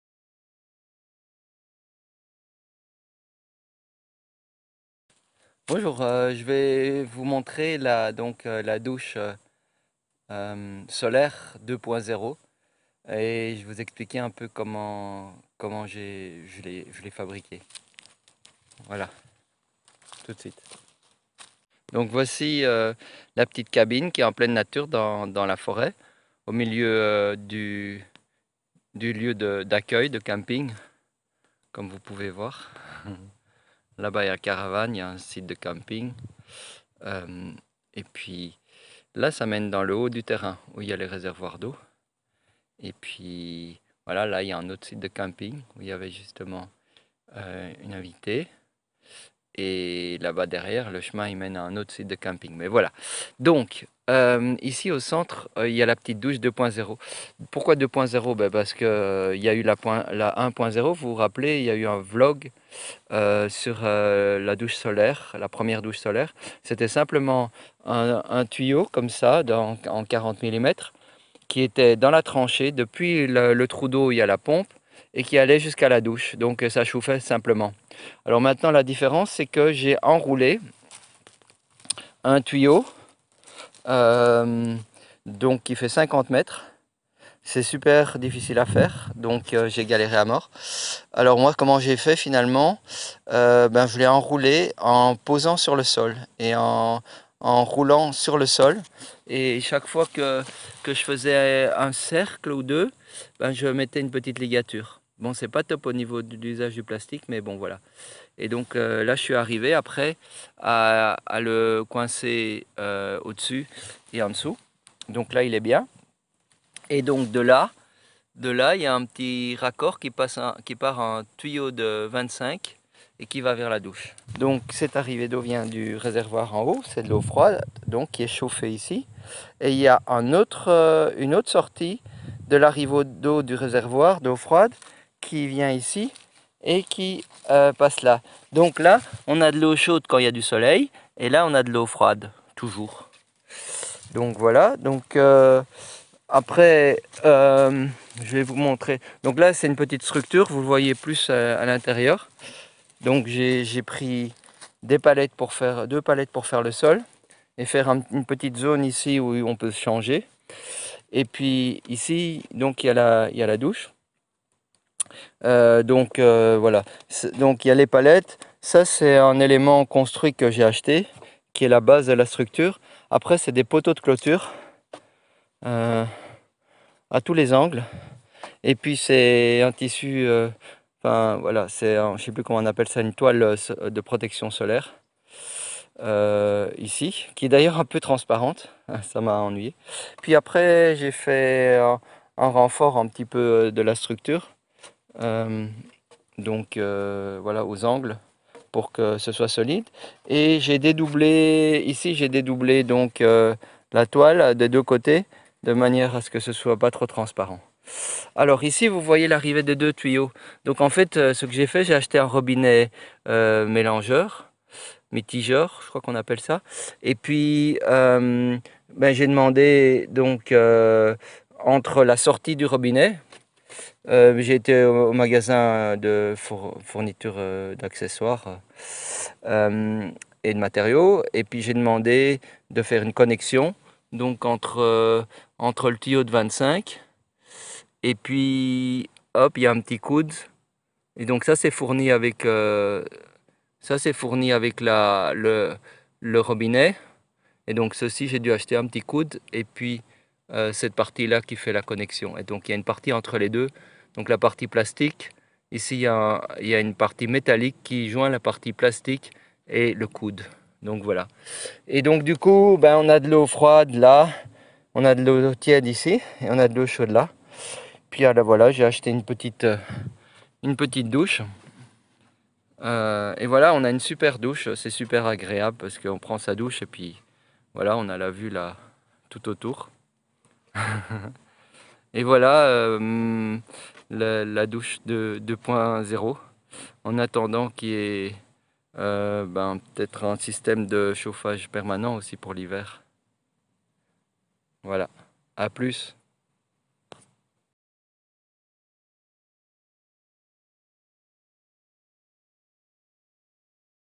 Portugal Terre Promise Vlog 33 Douche Solaire 2.0
Partage de mon projet et chemin d'installation au Portugal.

Un publi-reportage sur le vif sous forme de vlogs.

Vlog 33 : Comment j'ai construit la douche solaire 2.0, une amélioration de confort par rapport à la première douche 1.0 (cfr Vlog 27)